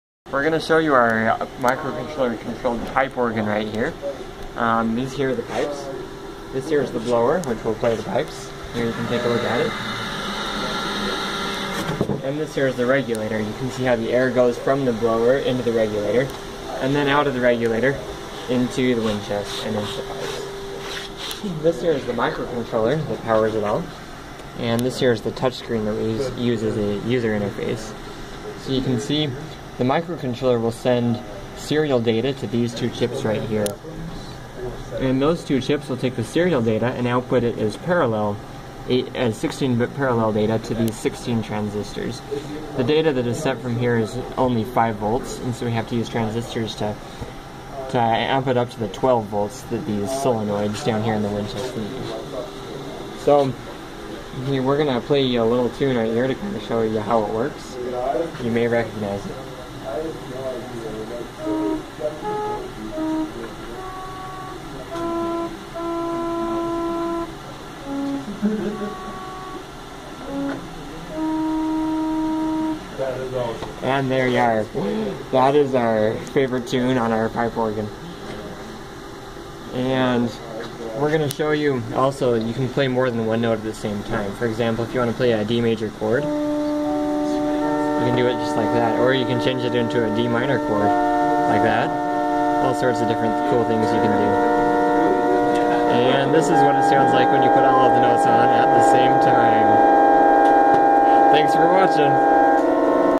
Microcontroller-Controlled Pipe Organ
This was a project for the class Microcontroller Hardware and Software. We took 16 pipes and hooked them up to a TM4C123GH6PM microcontroller. We used a touchscreen as a keyboard. Enjoy!